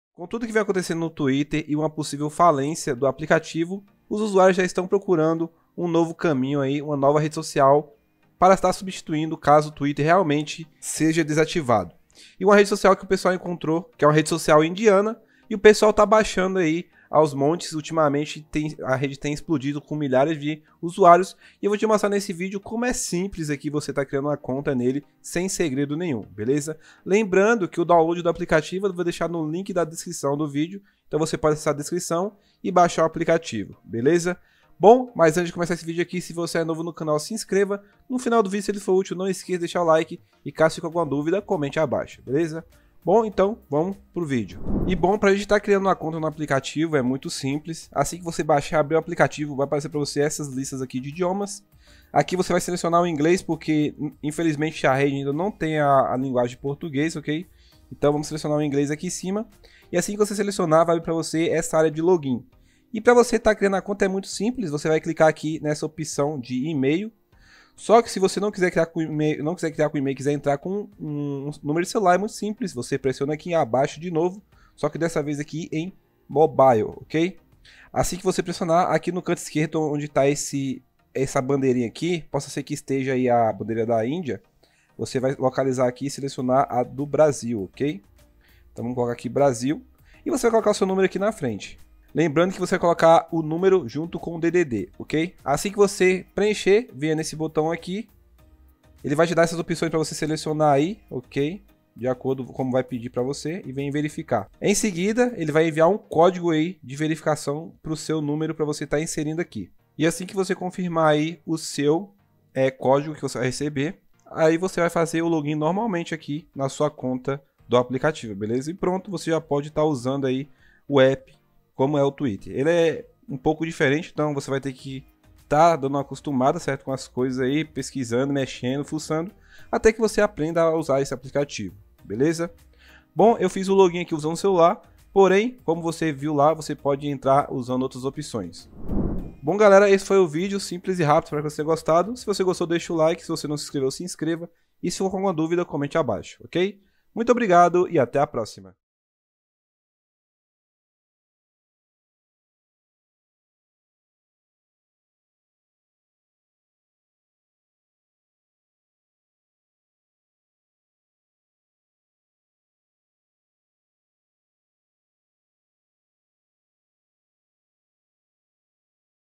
KOO
Com incertezas da continuação do Twitter, usuários começam a baixar o aplicaitvo Koo.